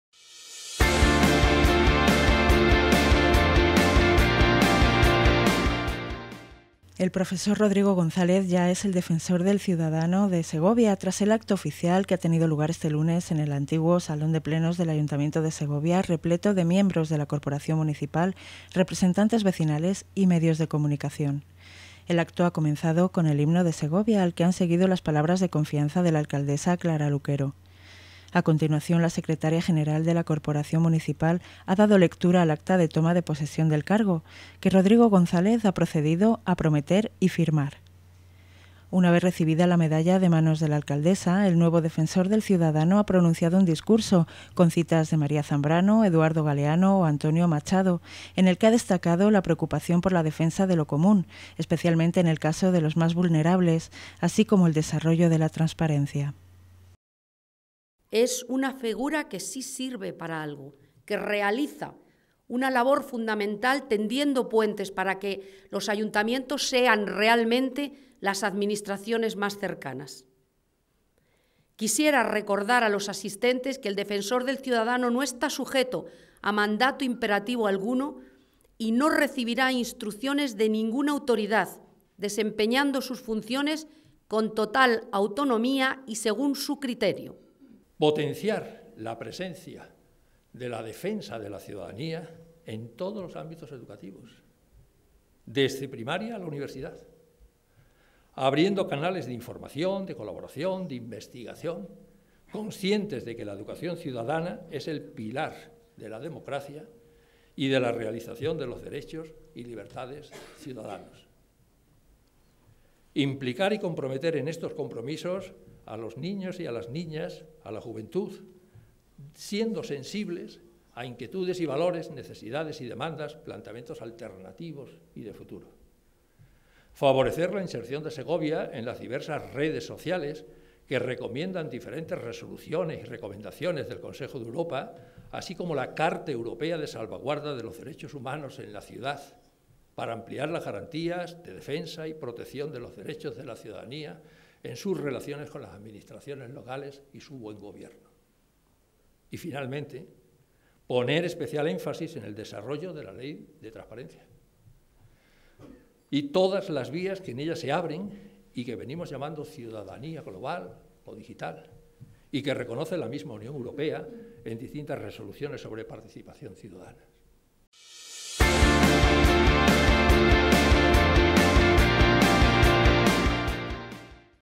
Rodrigo González destaca la transparencia en su toma de posesión como Defensor del Ciudadano
El profesor Rodrigo González ya es el nuevo Defensor del Ciudadano, tras el acto oficial que ha tenido lugar este lunes en un antiguo Salón de Plenos del Ayuntamiento de Segovia, repleto de miembros de la Corporación Municipal, representantes vecinales y medios.